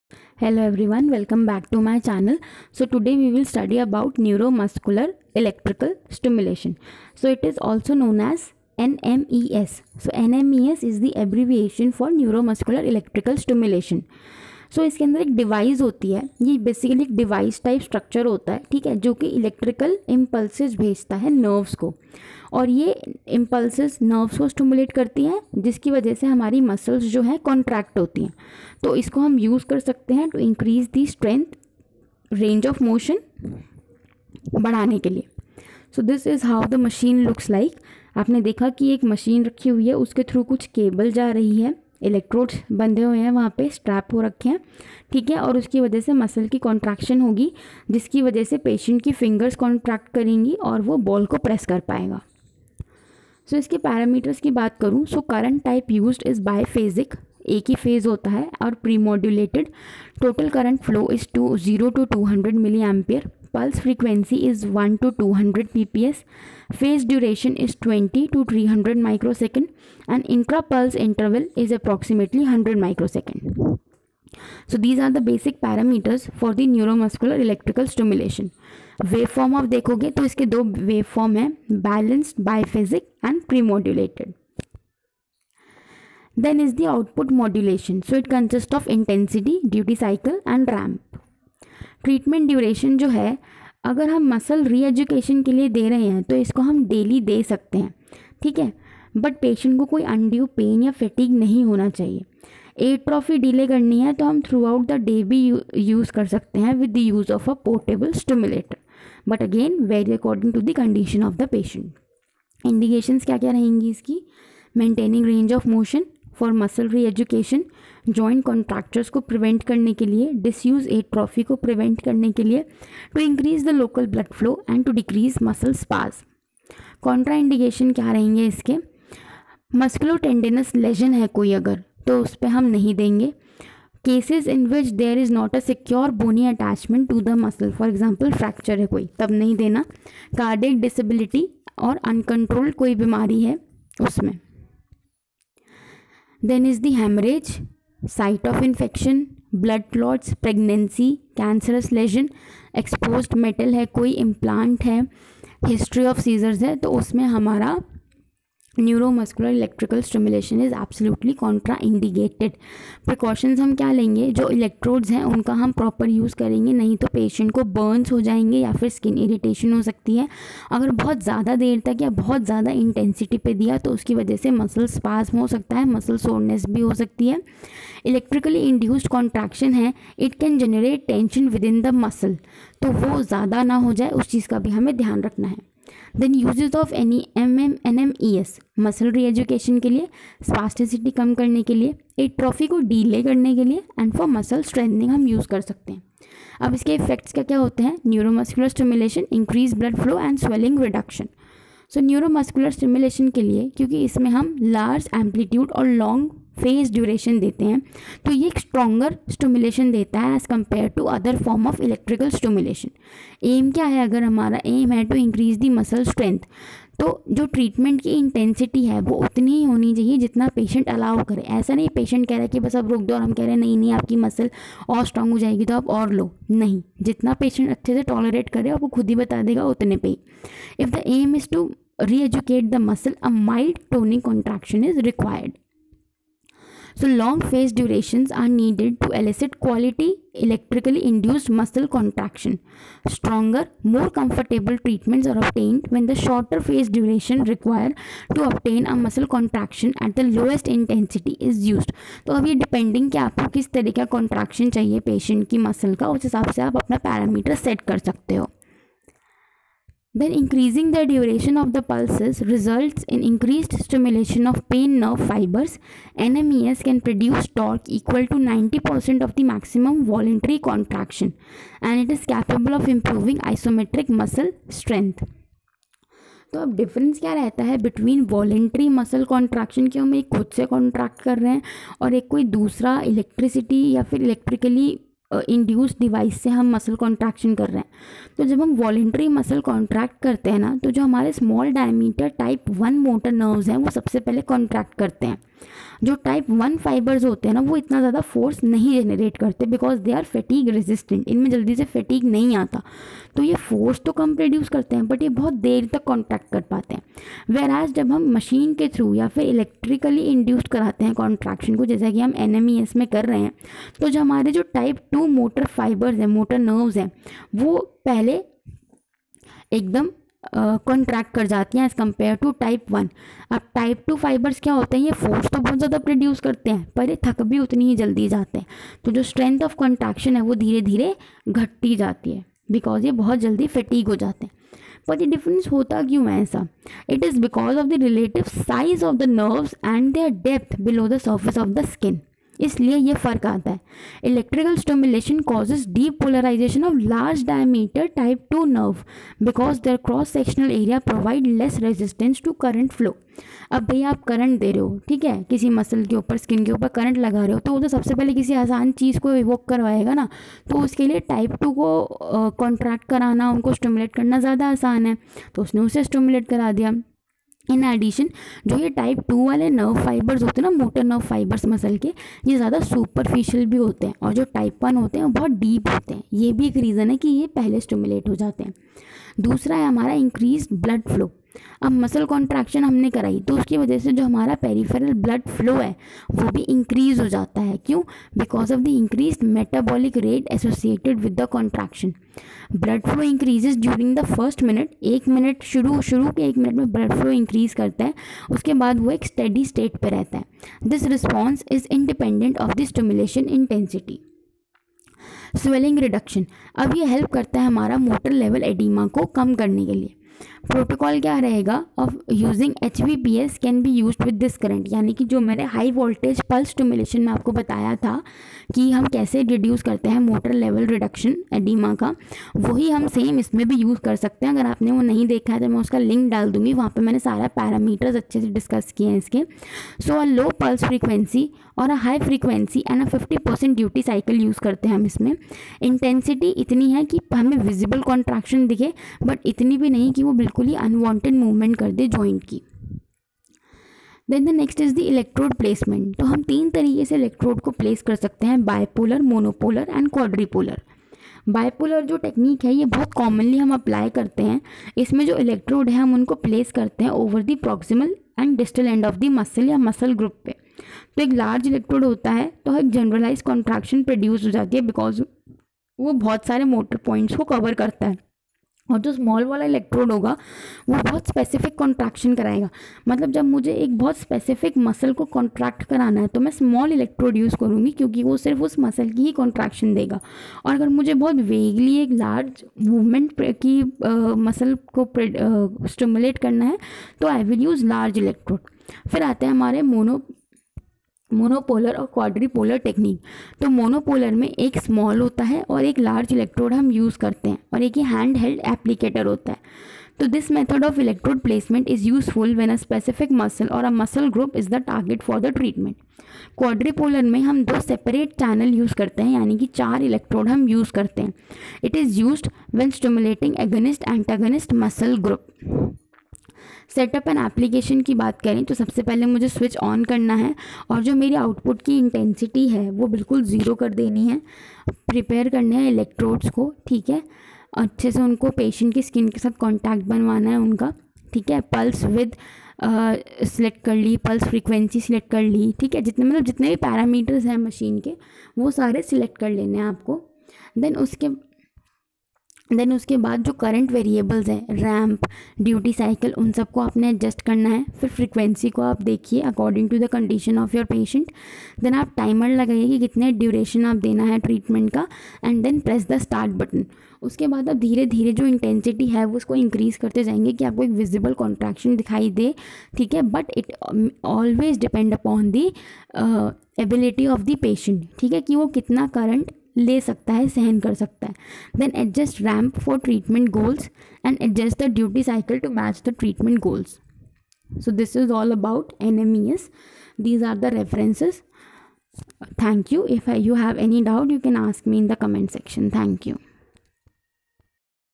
NMES- Procedure, uses, effects etc. with notes.
In this video I have talked about neuromuscular electrical stimulation (NMES).
If you have any doubt, please ask in the comment section, I will answer it to the best of my knowledge.